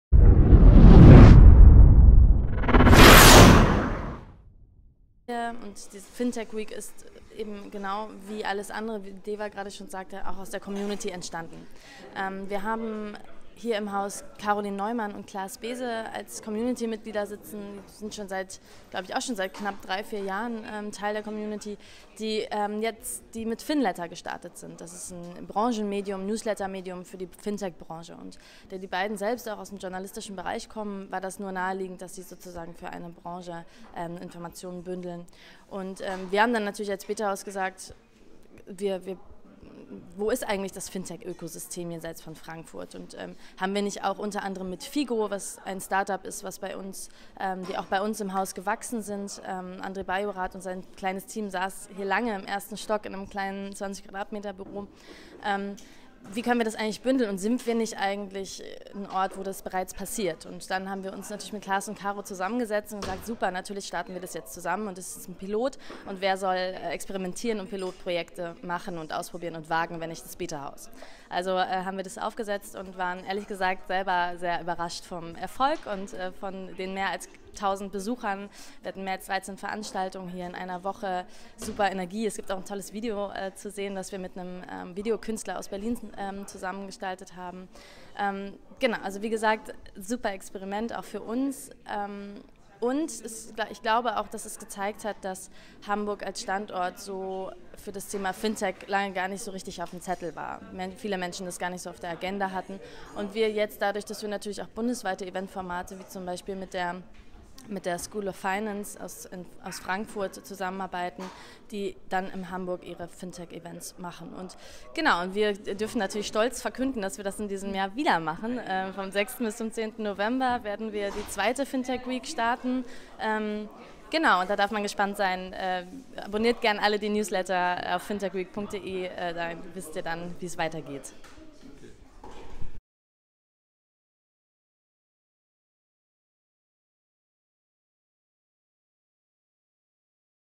Fintech Week Betahaus
Für den MOOC "Leuchtfeuer 4.0" habe ich das Team im Betahaus Hamburg zur Fintech Week 2016 befragt.